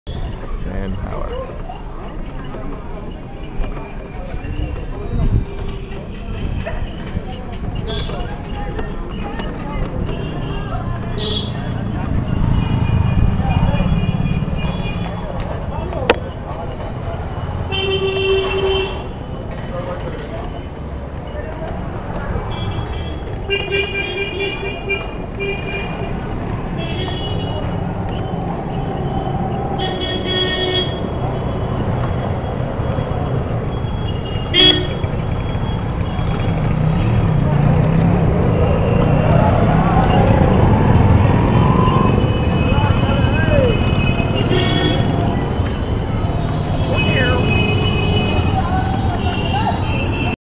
Bike Rickshaw ride in Varanasi
with a harmonium in my lap